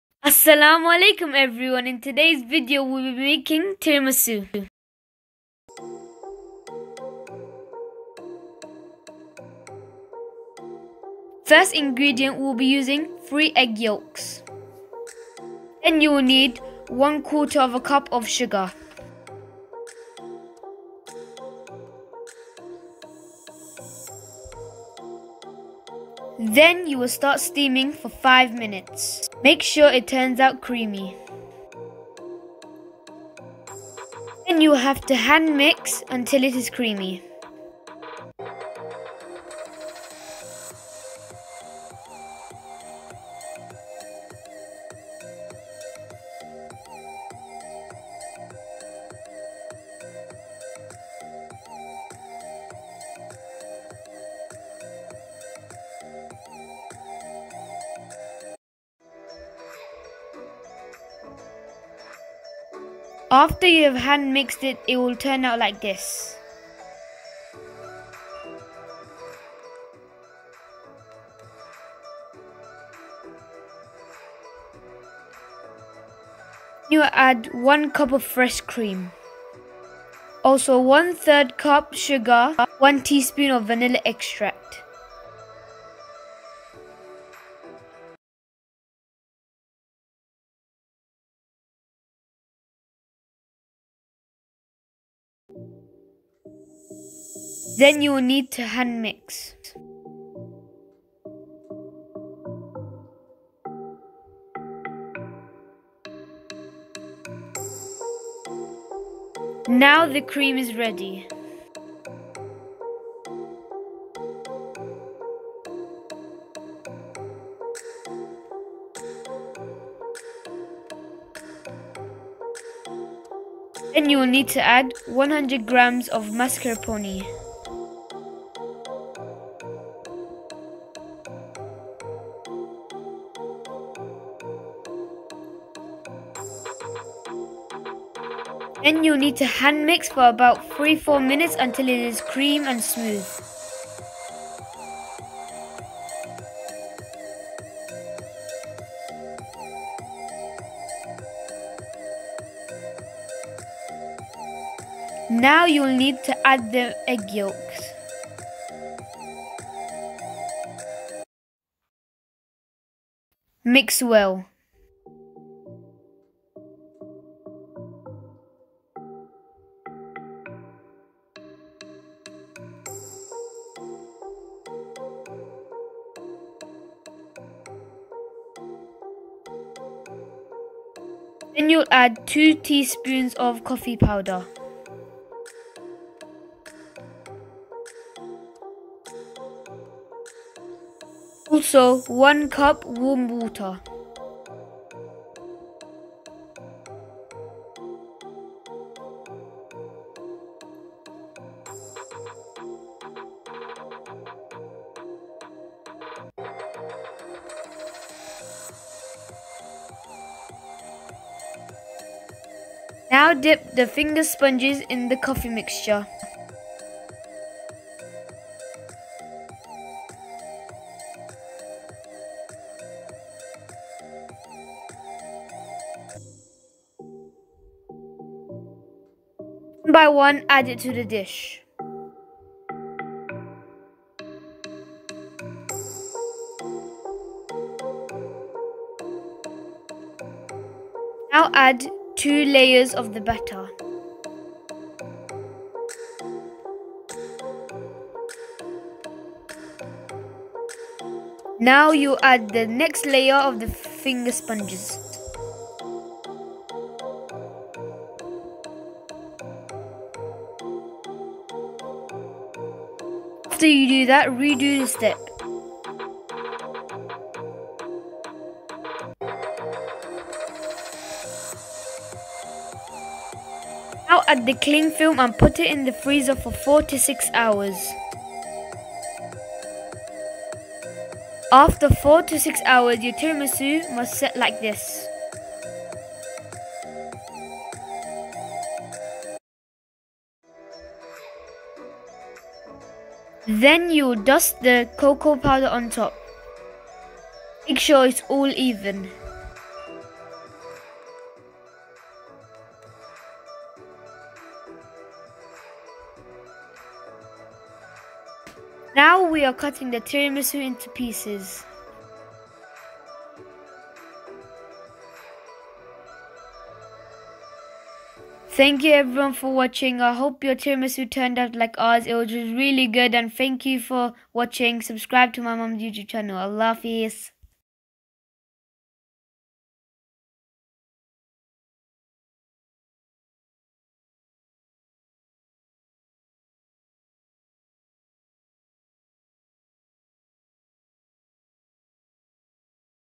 tiramisu recipe/cooking with lisa 💞
tiramisu recipe.
egg yolks 3
sugar 1/4 cup mix well
 1 cup whipped cream
1 /3 cup sugar
vanilla extract 1tsp
& mix well
add mascarpone 100 gm
& mix well
1 pack finger spang
coffee powder 2 tsp
water 1 cup
1 tablespoon  coco powder for dusting...